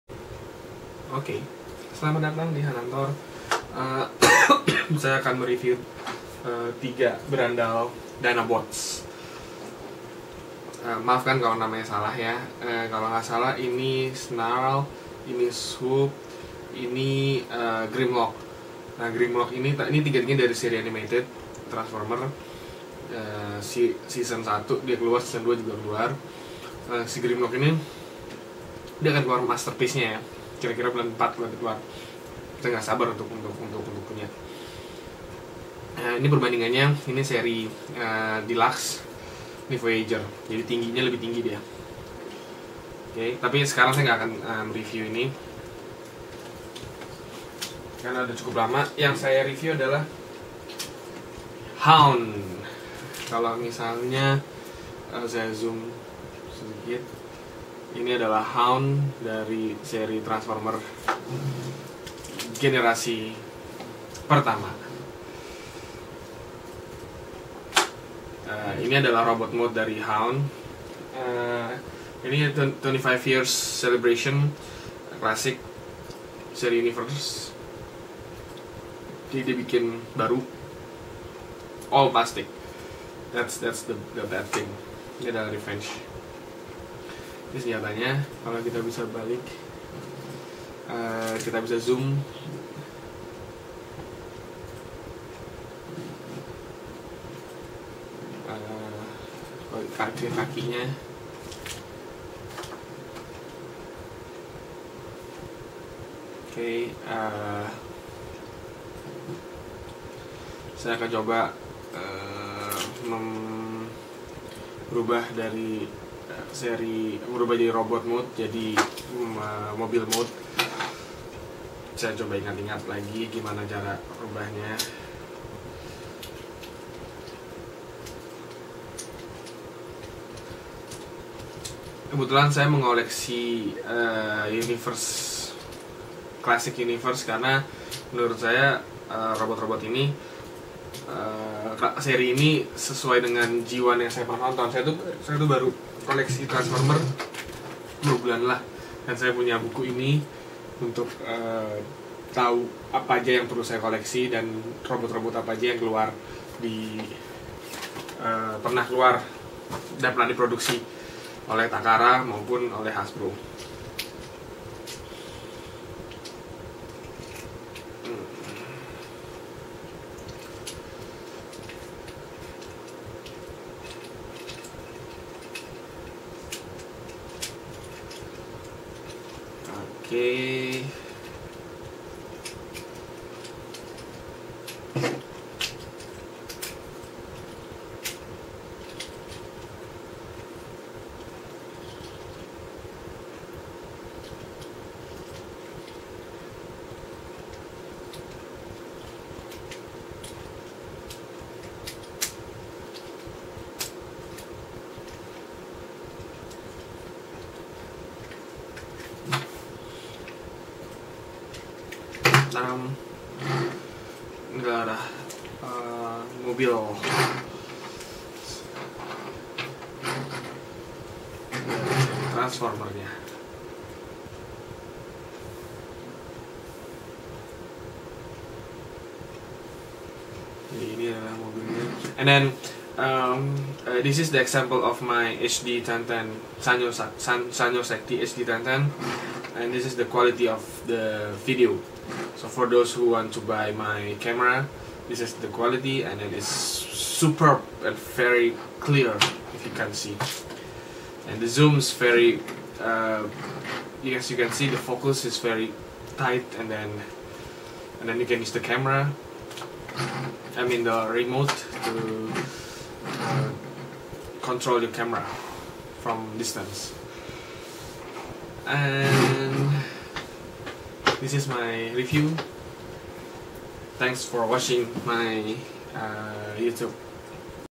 Transformers Hound
THis is my first review using Sanyo xacti HD1000. This camcorder is great for Youtube and the quality is superb in HD mode.